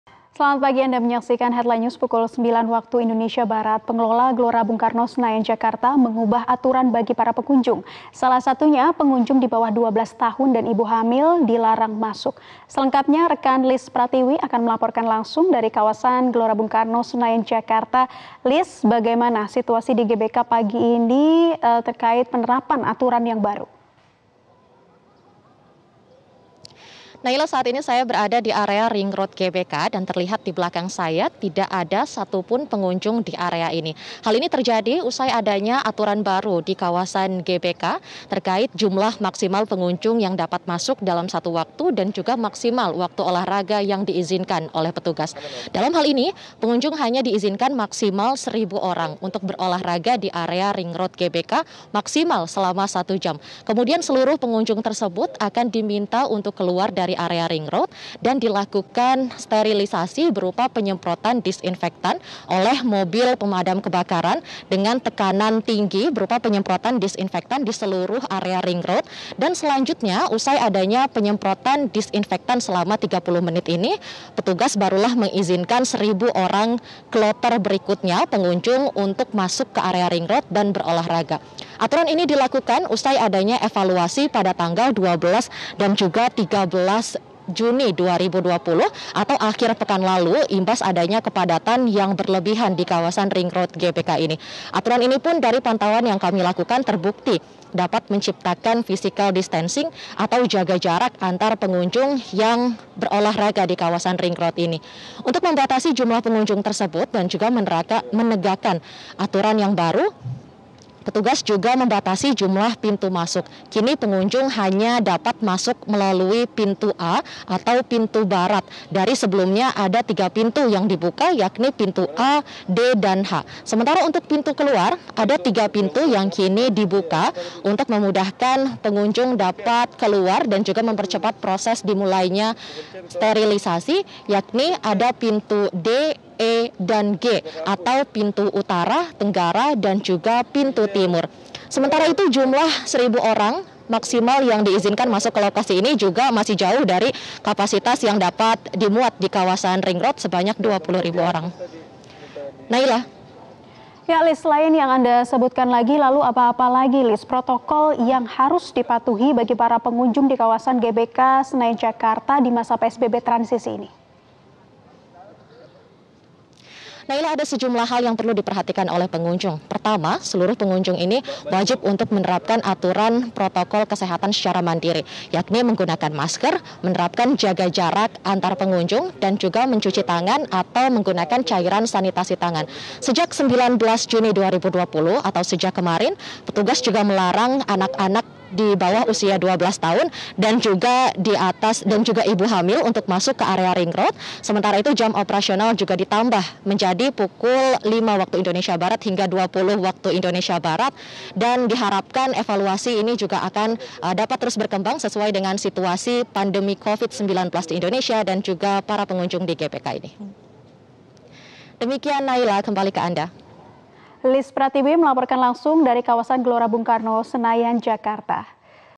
Aturan Baru GBK, Anak anak dan Ibu Hamil Dilarang Masuk
Kawasan Gelora Bung Karno (GBK) menerapkan aturan baru bagi masyarakat yang hendak berolahraga di masa PSBB transisi. Aturan ini terkait pembatasan jumlah pengunjung yang mulai diberlakukan hari ini, Sabtu (20/06/2020). Pengunjung di bawah 12 tahun dan ibu hamil dilarang masuk.